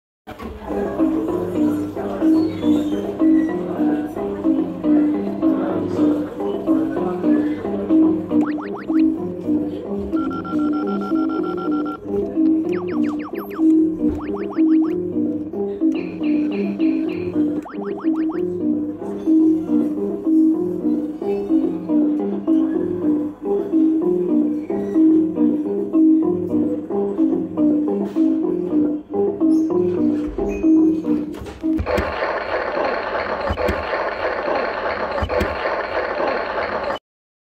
Toy Shop - Music and sound effects (also used on rpresas machines)
These claw machines are usually seen in restaurants or pubs in the uk. these machines are listed on the maxx grab

a company called RPresas uses similar sounds on their machines such as Clawsaurus, Super plus, etc.

(from maxx grab website):
Toy & Candy Shop is the smallest commercial toy crane available! Now available in 2 different sizes. Perfect for kiddie play areas and venues that need machines with a small footprint. Spectacular artwork and a "shop window" effect to try to entice customers to play!